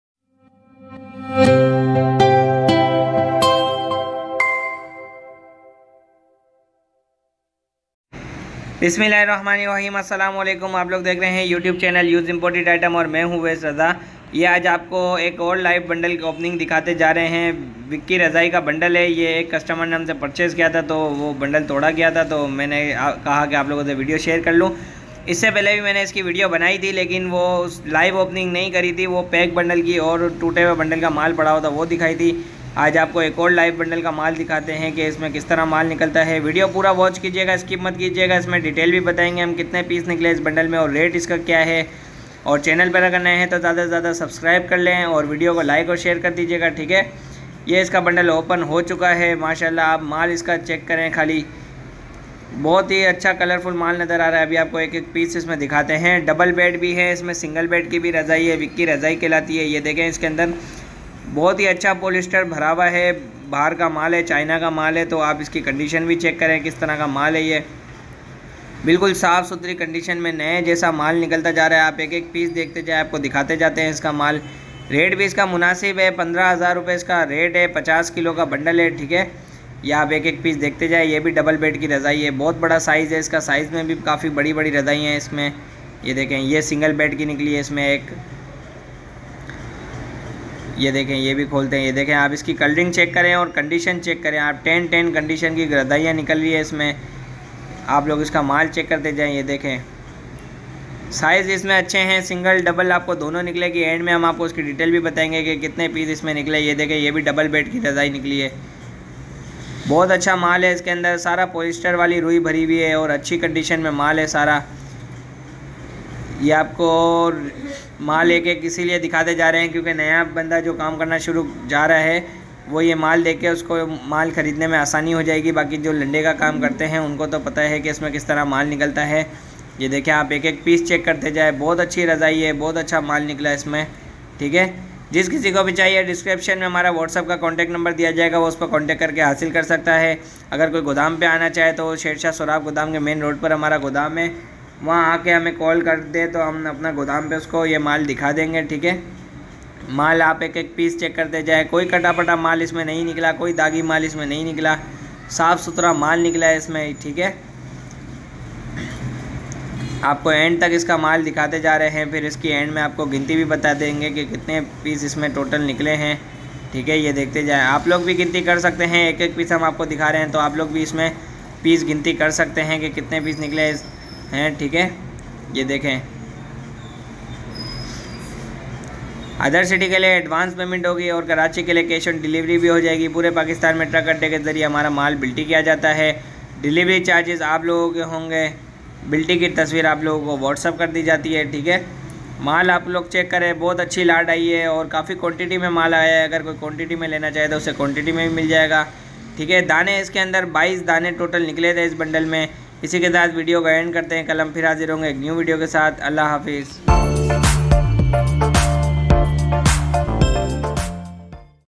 China vicky razai ka A++ stock 50 kg ka bundle sherhah landa market karachi
sherhshah landa market karachi | vicky razai | used imported items

as salam u alaiekum yeh hamare pas china imported vicky razai aye hain 1 bundle 50 kg ka  lena hoga jis kisi ko bhi chahiye wo whatsapp per contact ker ke hasil ker sakta hai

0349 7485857
OWAIS RAZA